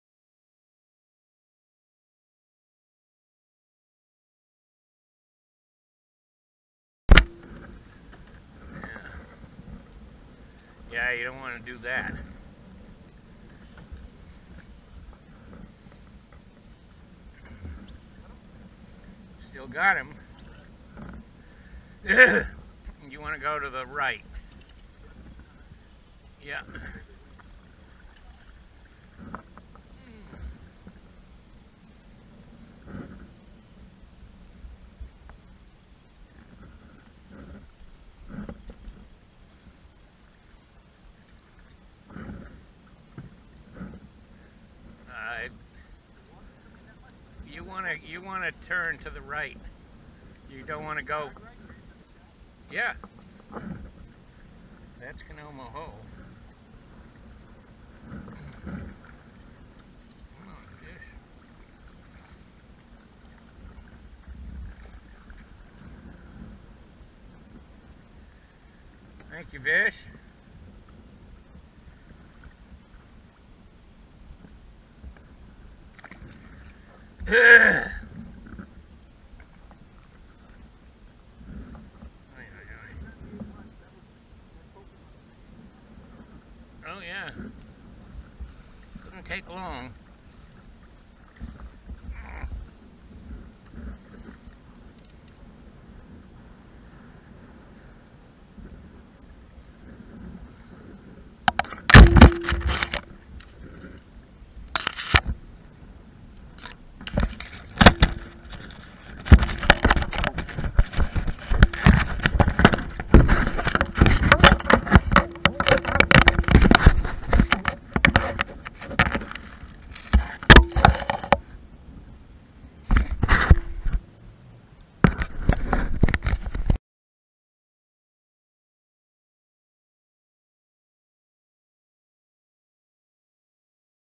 20200925 FBJ VIDEO #14 -- Fred gets his 22nd fish in the Essex River on 25 September 2020 at 1:51PM
We left Outlet Hole and paddled downriver through ByPass Hole and the Y Pool and then down through Conomo Hole and into the Essex River without even a strike, so we stopped along the Essex River to have a bite to eat and wait until the incoming tide started to run well. This video starts after all that, just as we are paddling up the Essex River about to turn back up into Conomo Hole, when I get my fish #22 on my fly. I bring it in and then we continue on upriver into and then through Conomo Hole. We decide that, instead of going back up the main river, we would paddle back up through the Y Pool and ByPass Hole and try out luck again at Outlet and Hog Holes, since that is where we had found so many fish on the ebbing tide.